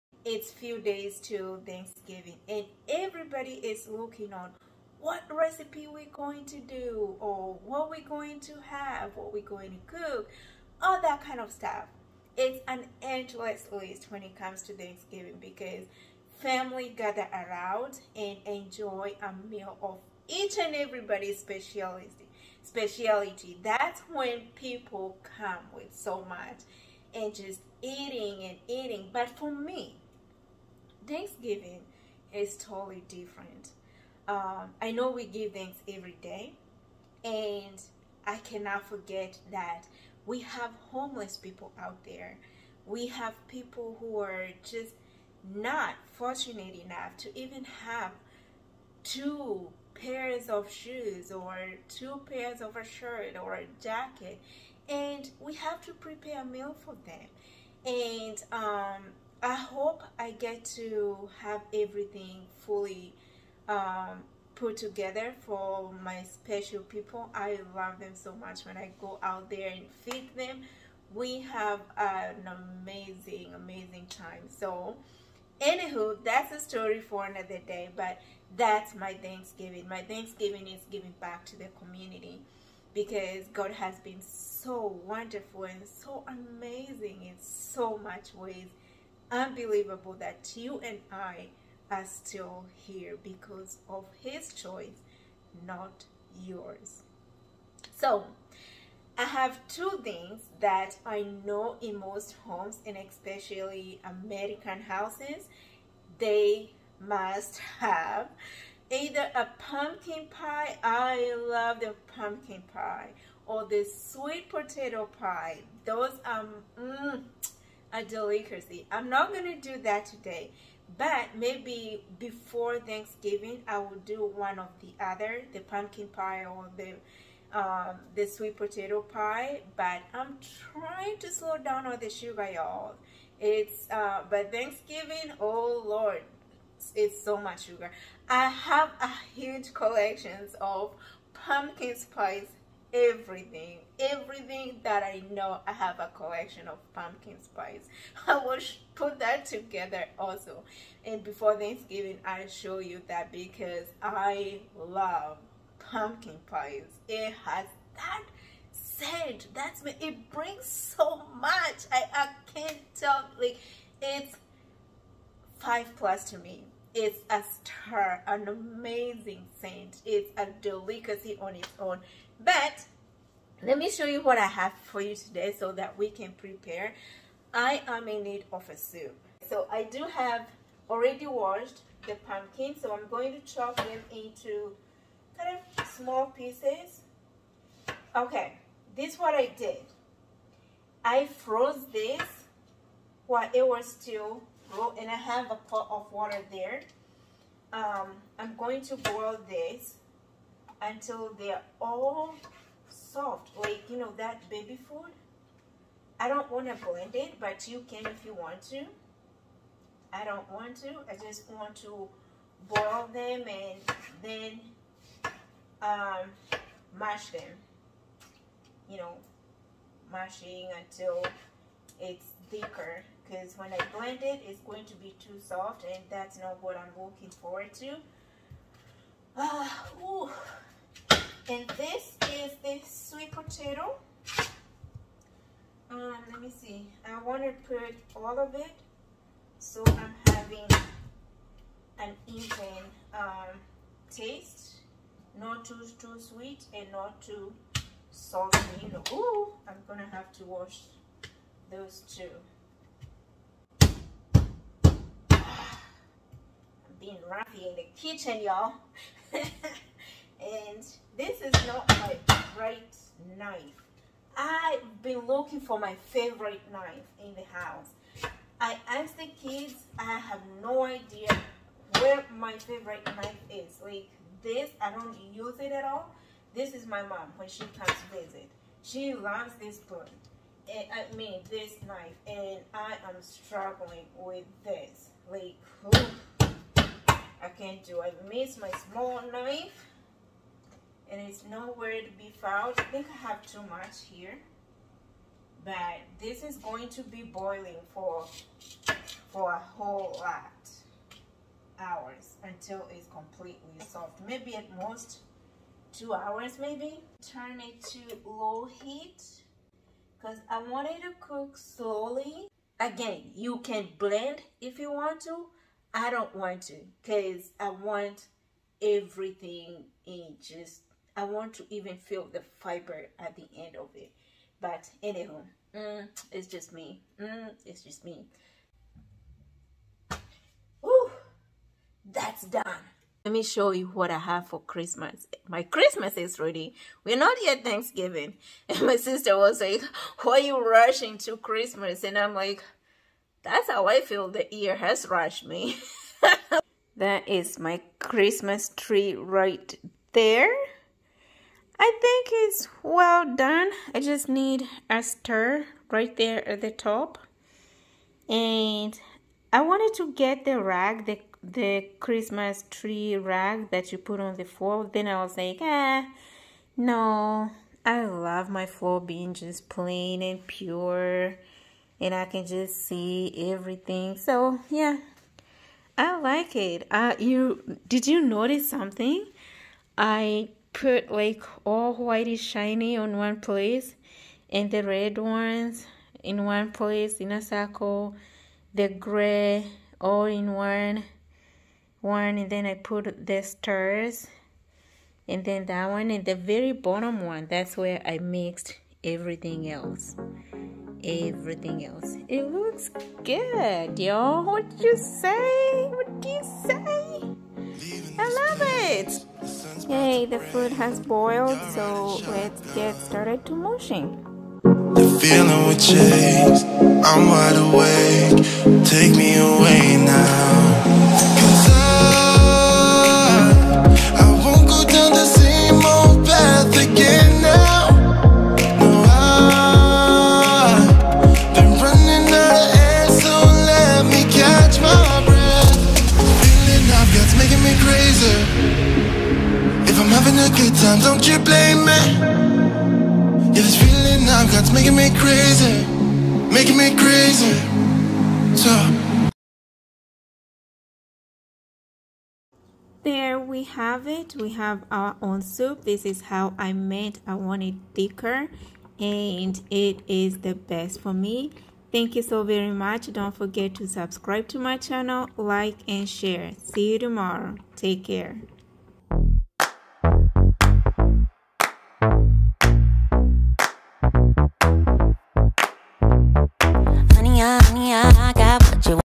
😚😚The simplest way on how to make a pumpkin soup 😋😋😋
This is how you make a pumpkin soup in the most simplest way.